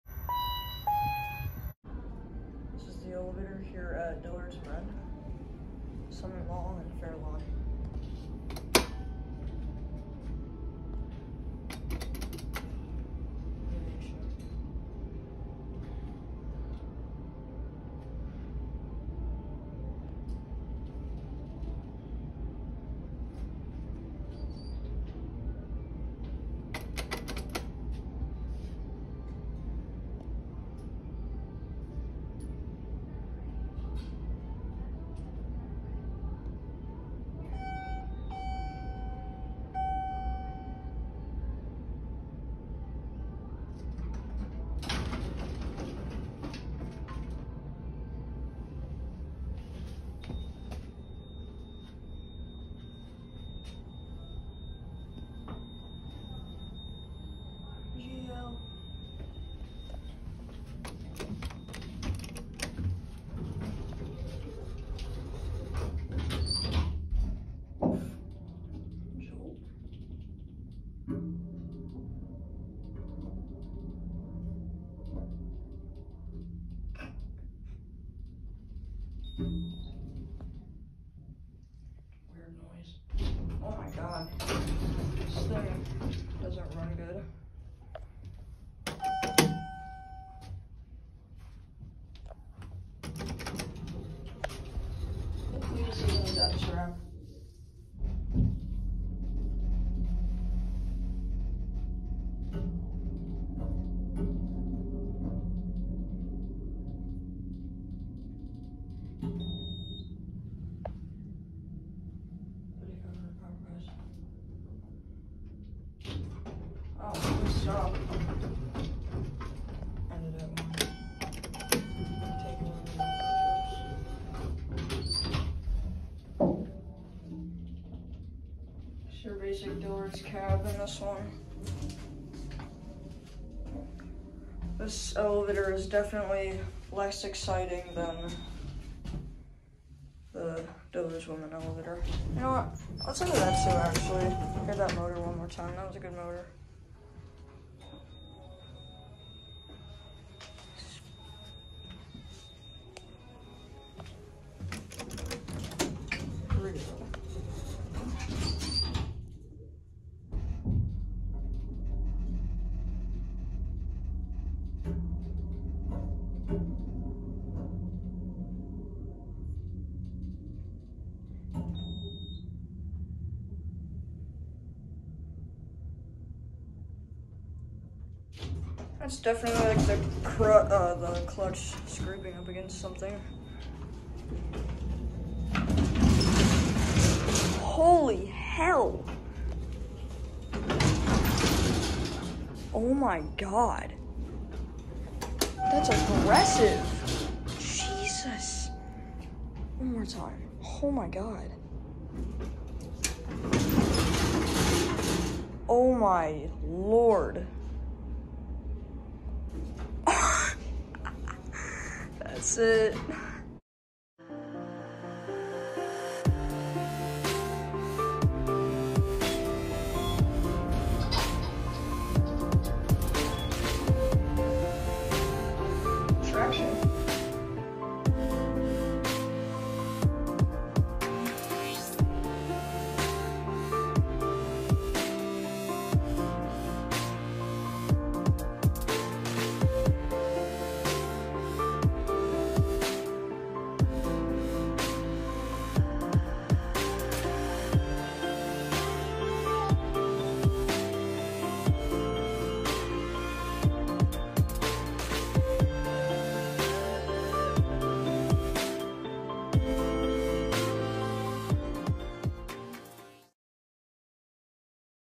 Lovely DOVER (mb. KONE) Hydraulic Elevator @ Dillard’s Men - Summit Mall - Fairlawn, OH
(10/11/22)
This elevator is very basic for a Dillard’s. This elevator runs like shit and the doors are terrible! The motor is nice, but the clutch or something is scraping everything in the shaft. For doing a great job modernizing the other Dillard’s elevator, I wonder why this one is so choppy.

Specifications:
Manufacturer: DOVER
Modernizer: KONE
Model: ?
Fixtures: Innovation Universal
Drive: Hydraulic
Capacity: 3500 lb
Year Manufactured: 1965
Year Modernized: early 2004